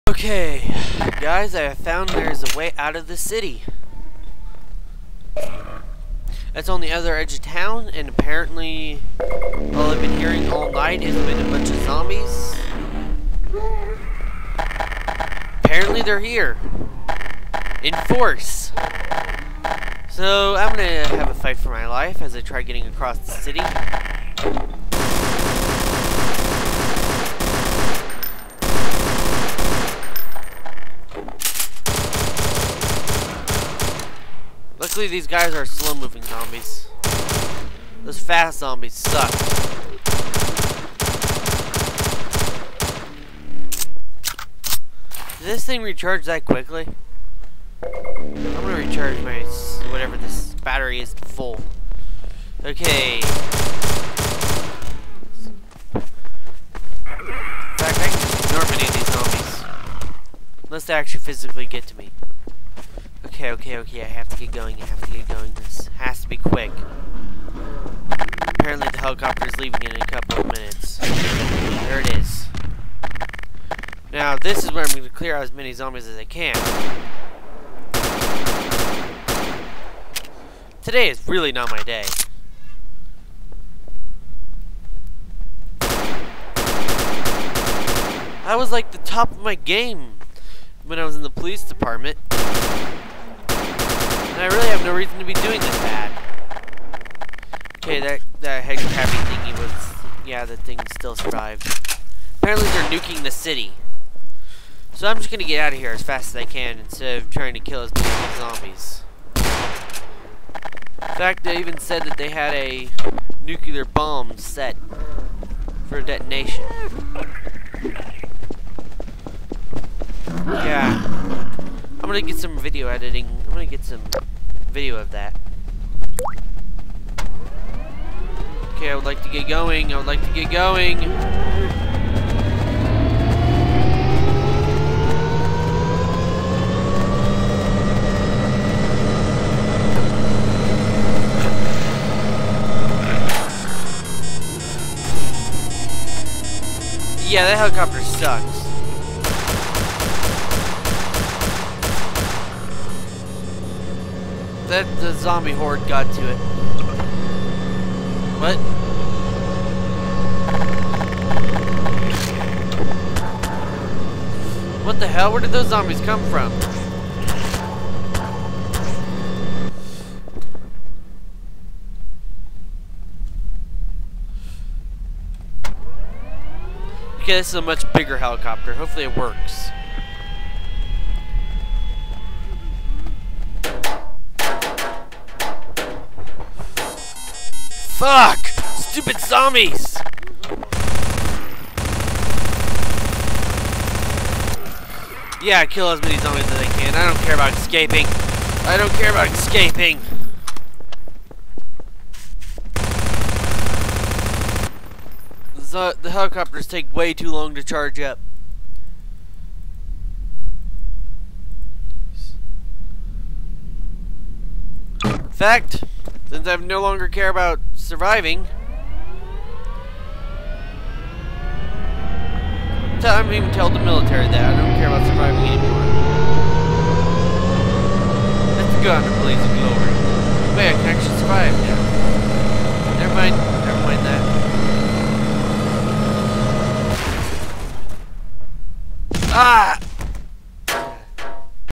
Part 8 the way out
i find a way out of the city but i die anyway because of map limits.